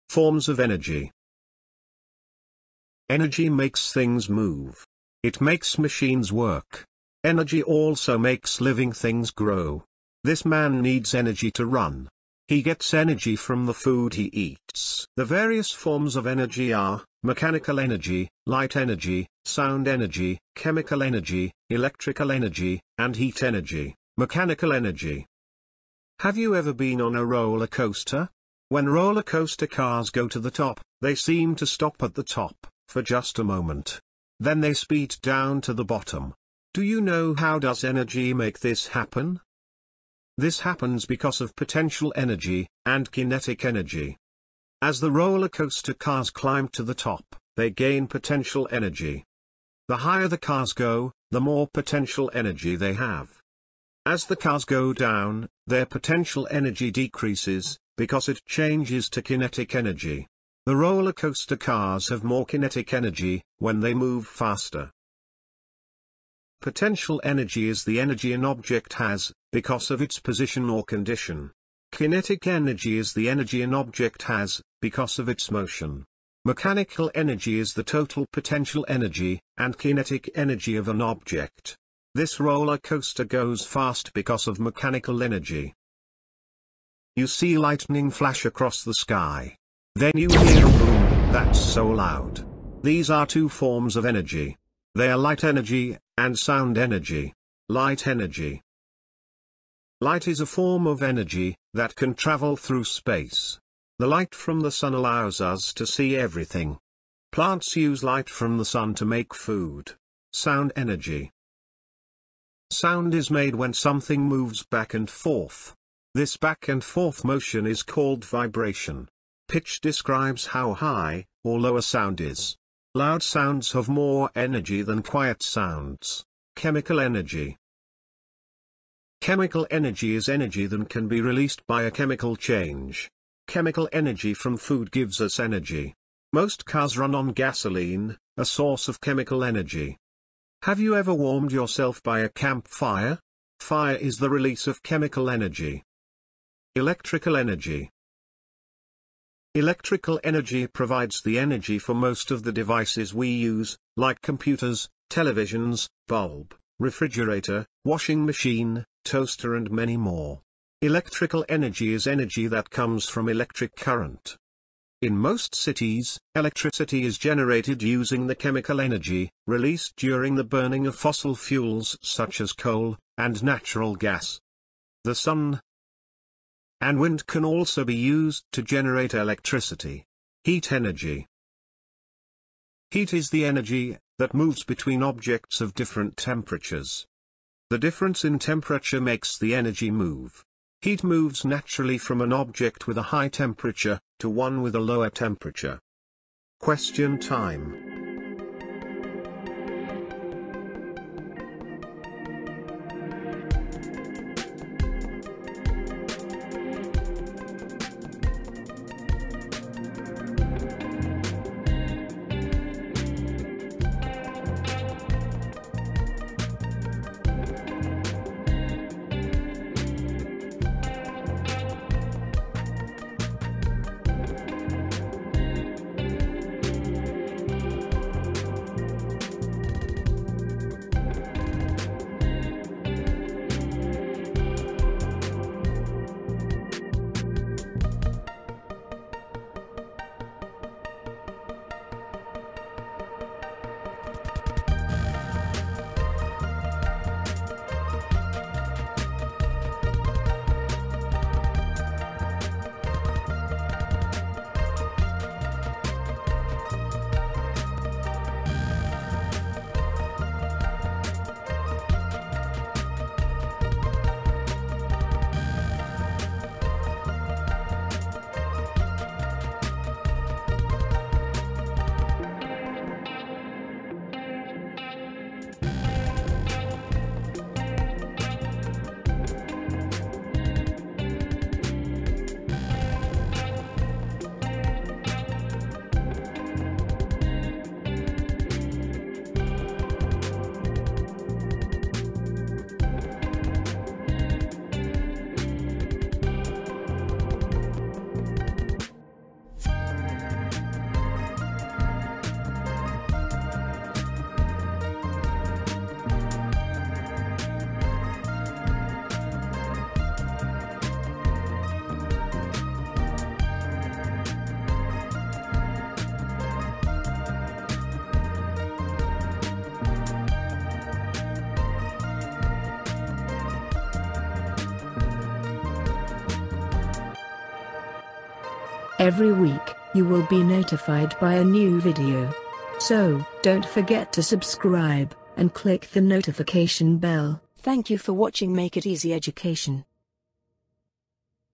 FORMS OF ENERGY || TYPES OF ENERGY || SCIENCE EDUCATIONAL VIDEO FOR CHILDREN
THIS VIDEO EXPLAINS ABOUT VARIOUS FORMS OF ENERGY. IT IS HELPFUL FOR CHILDREN.